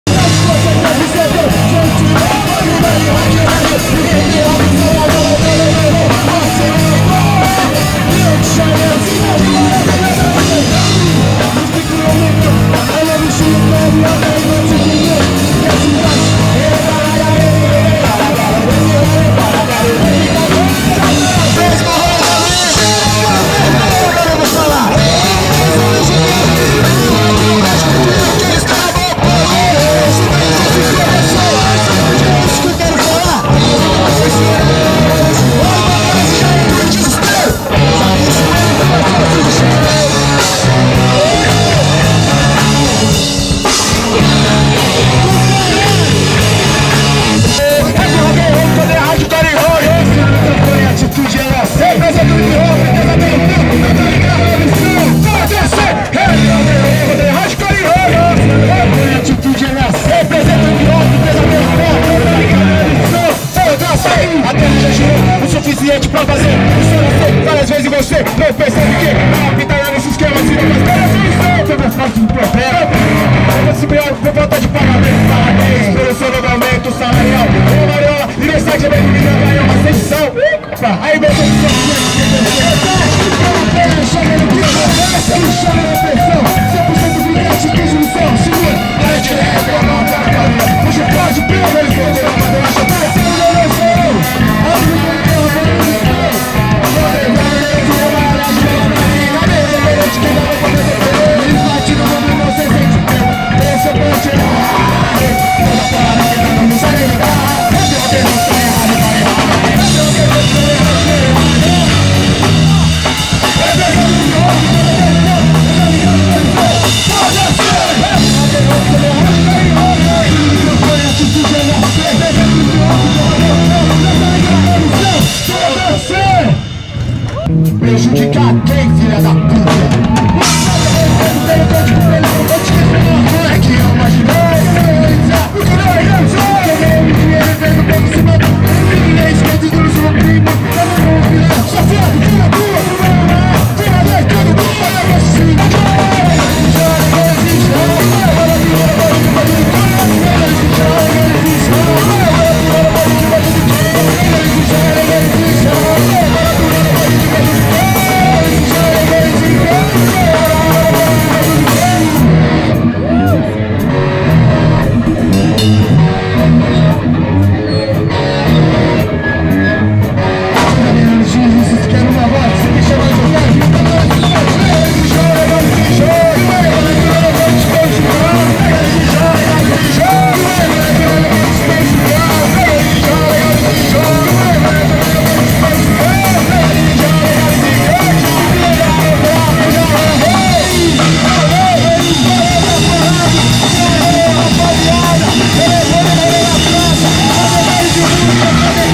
T.H.Cificados no Mercadão
Evento Independente organizado pelo Coletivo Arte e Rua.
Mercadão Municipal de Maringá/PR
13/09/2014